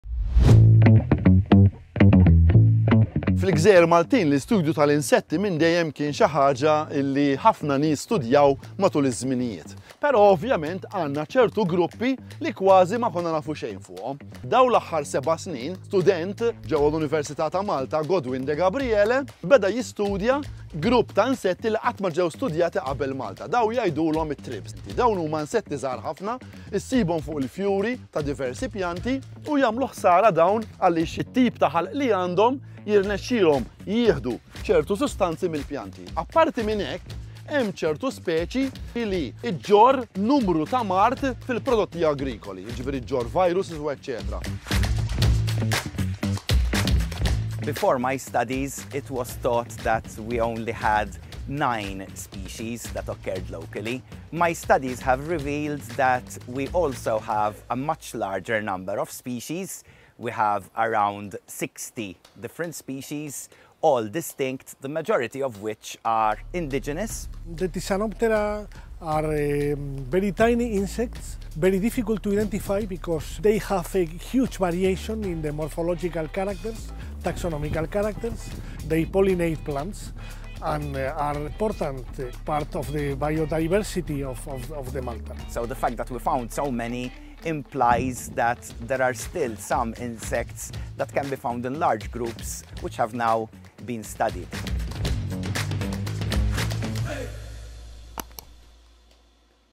Studying Thrips | Thysanoptera
PhD student, Godwin Degabriele (under the supervision of Professor David Mifsud), has discovered 60 species of thrips on the Maltese islands. These small insects can be found in flowers of several plants and may pose a threat to agricultural products.